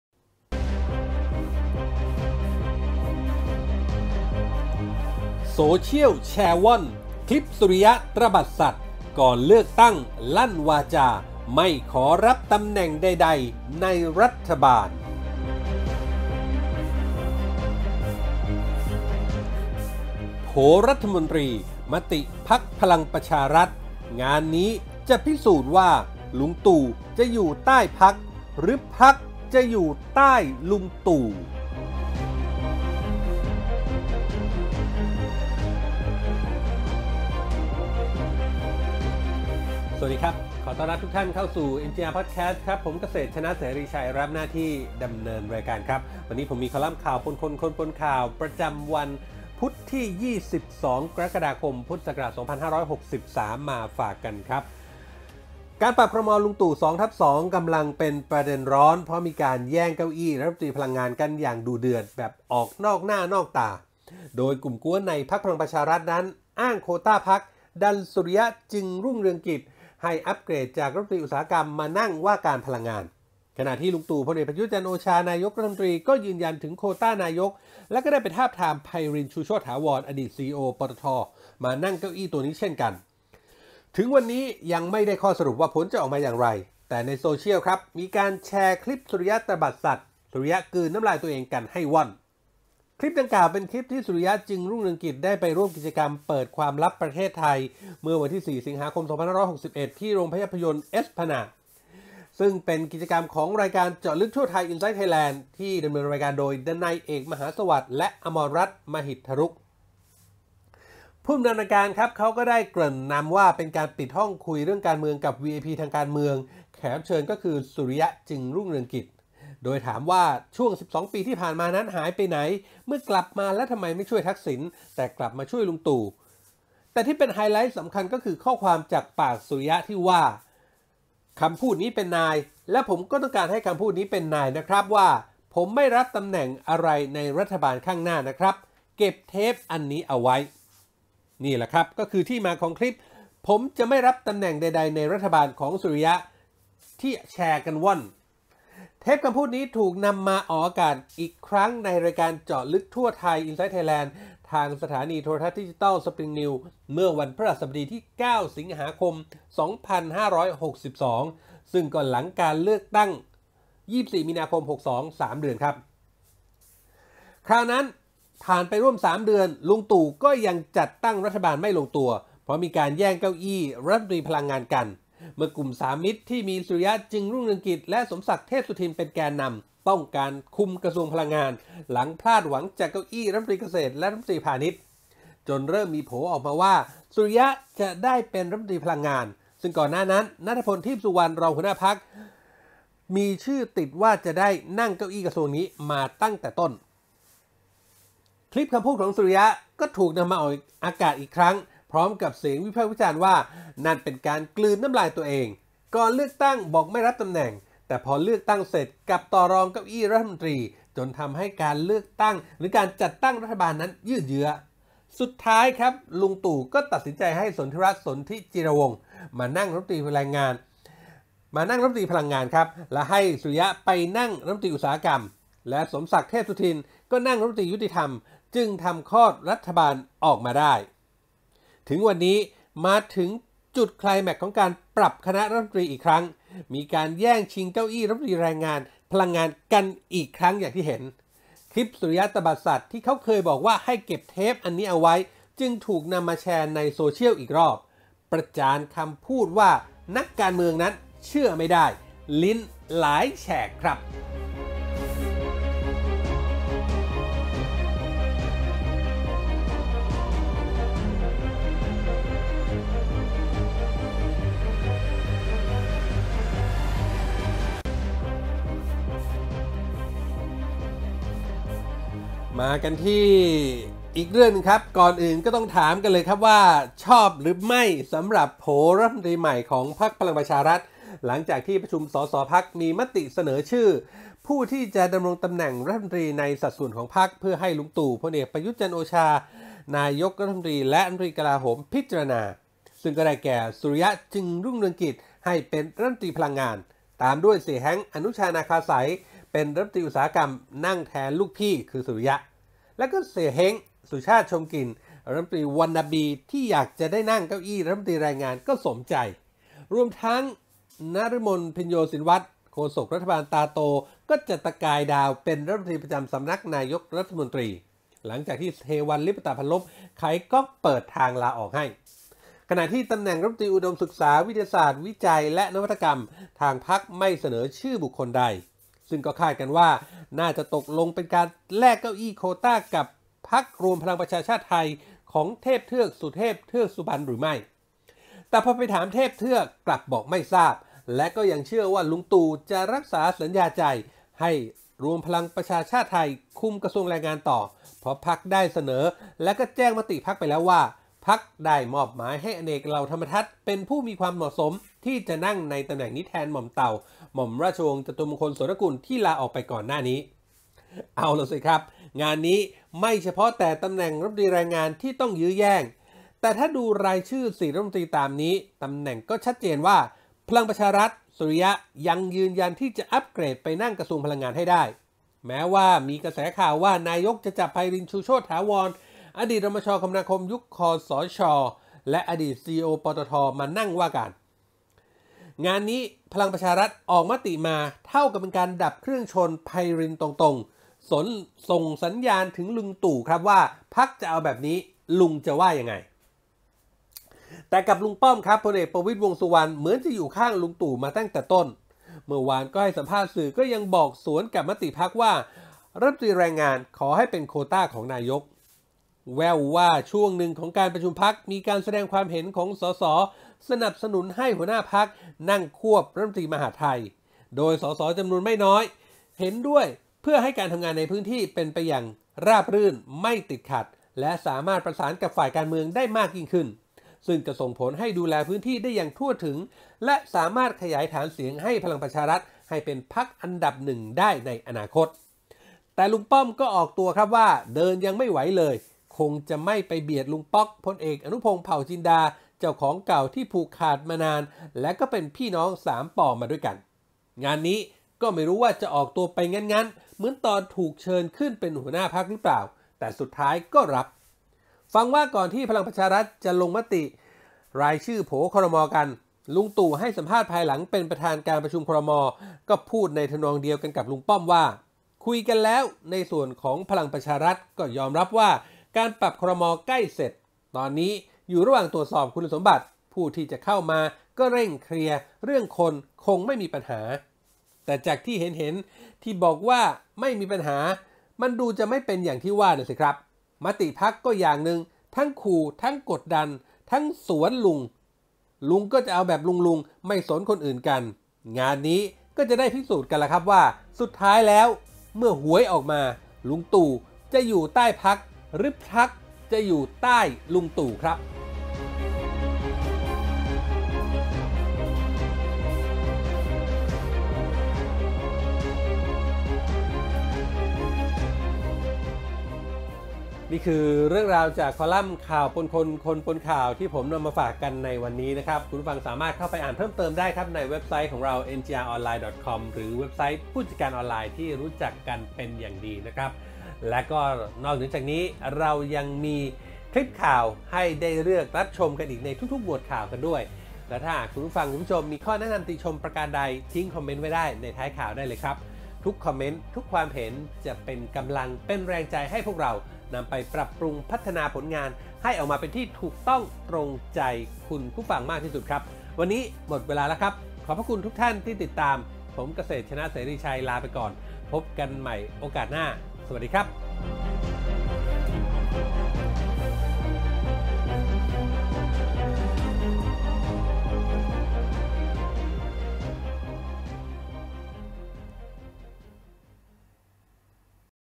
โซเชียลฯแชร์ว่อน คลิป “สุริยะตระบัดสัตย์” ก่อนเลือกตั้งเคยลั่นวาจา ไม่รับตำแหน่งใดๆในรัฐบาล
โซเชียลฯแชร์ว่อน คลิป “สุริยะตระบัดสัตย์” ก่อนเลือกตั้งเคยลั่นวาจา ไม่รับตำแหน่งใดๆในรัฐบาล / โผรัฐมนตรี มติพรรค พปชร. งานนี้พิสูจน์ว่า ลุงตู่จะอยู่ใต้พรรค หรือพรรค จะอยู่ใต้ลุงตู่